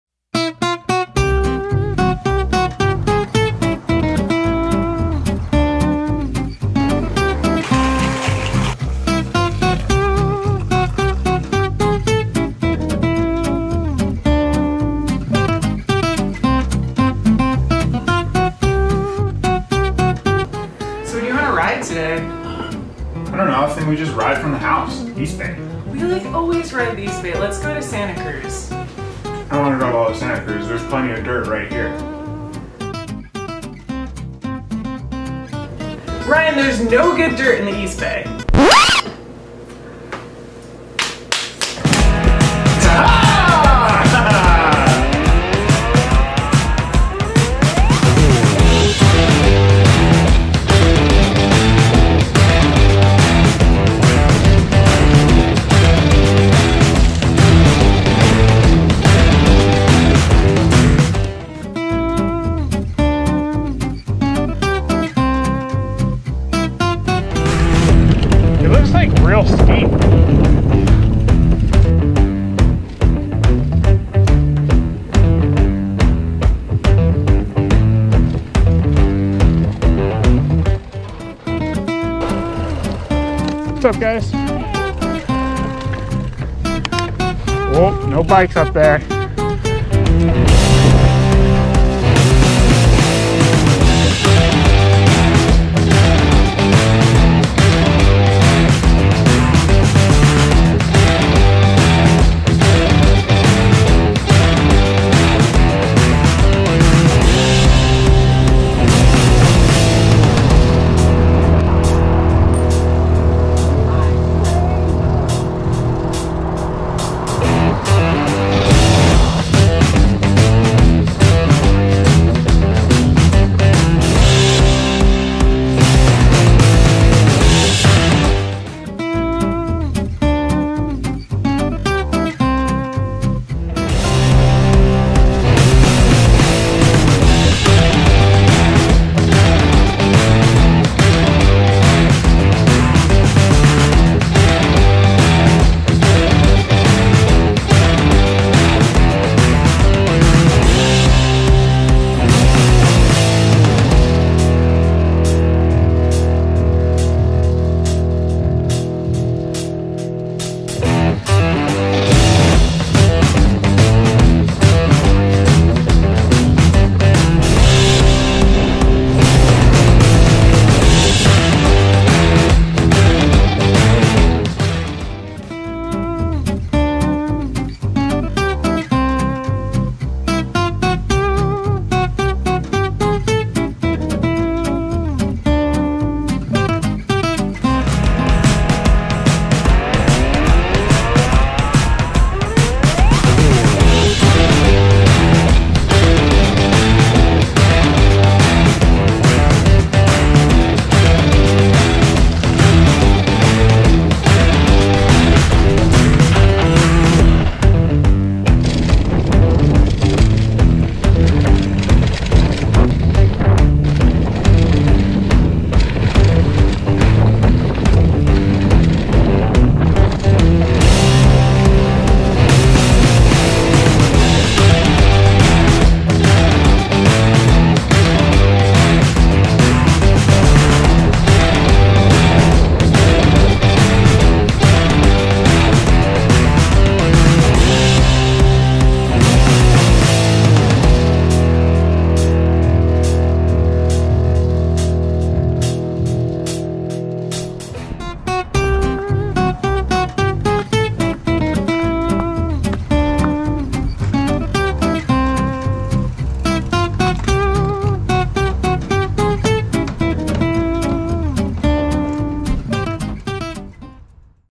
Ryan's Day
Brother and sister Ryan and Becky have very similar riding styles and interests. Most of the time, they see eye to eye on the day's riding plans. But just because they have matching bikes doesn't mean they always agree on where to ride. When it comes to siblings, sometimes you win, and sometimes you go on a 100-mile death march through Northern California.
You win this round, Ryan...